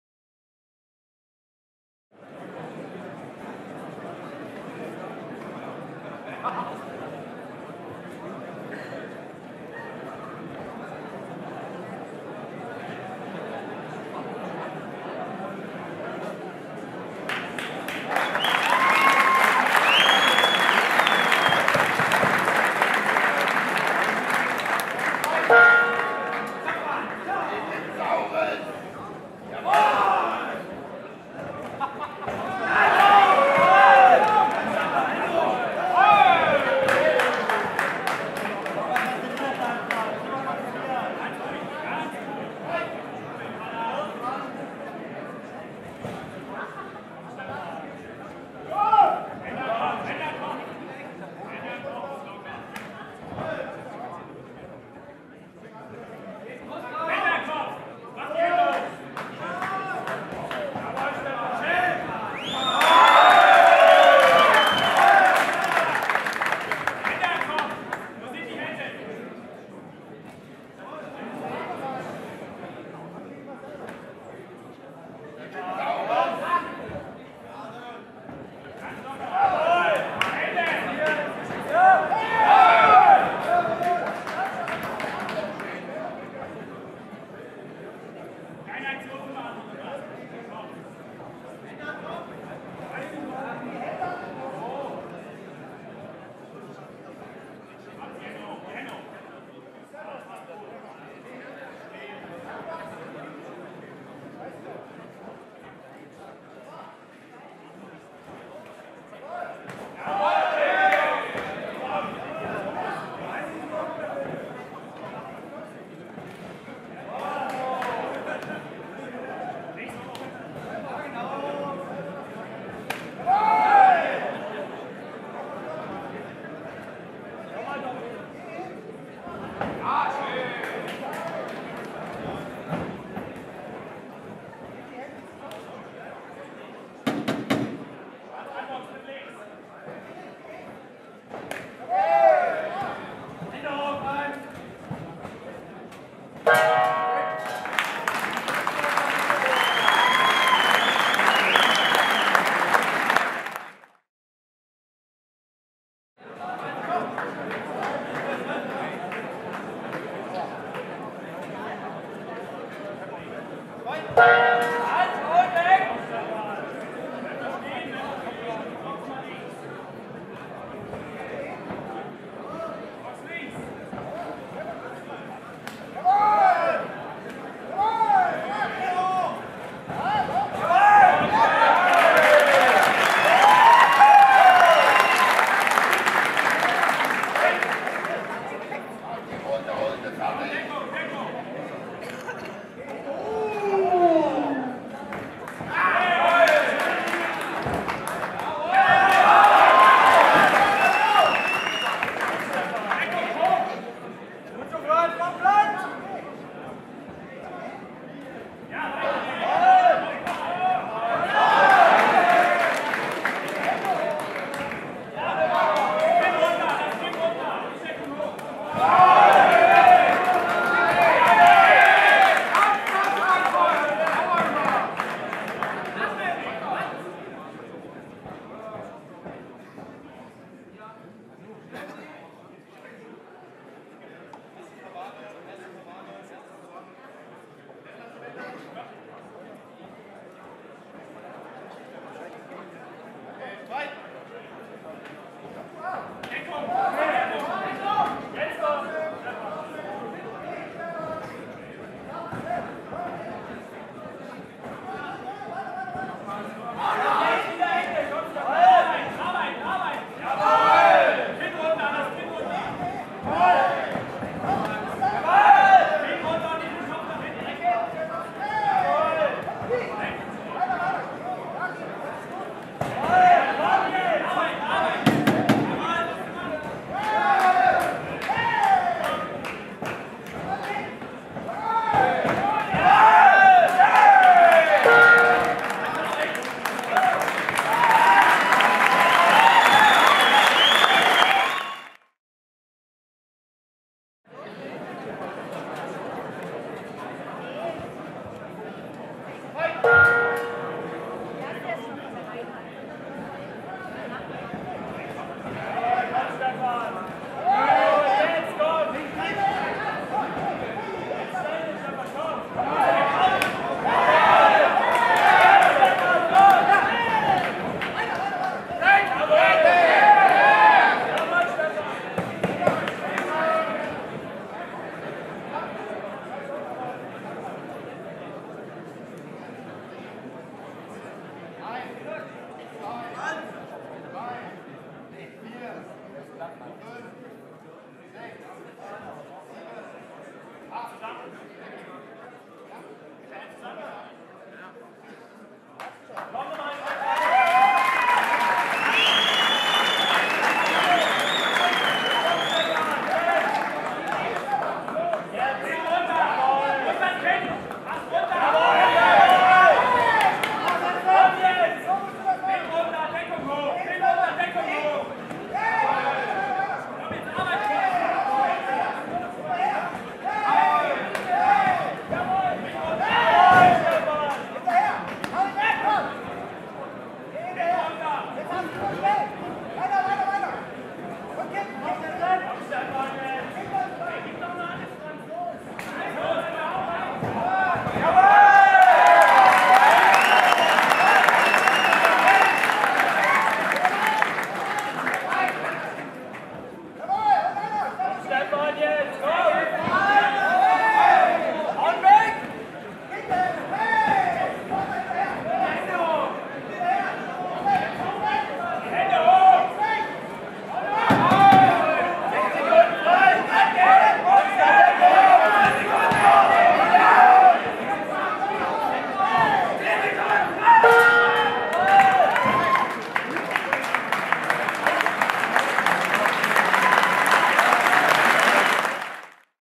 Ironsports Fightnight 03.12.2011 - Fight 2: Franz vs. Stefan
Rot - Franz Ironsports vs. Blau - Stefan Team "Spirit of Bayon"
K1 Rules
3x2min

Ergebnis: Sieg nach Punkten - Blau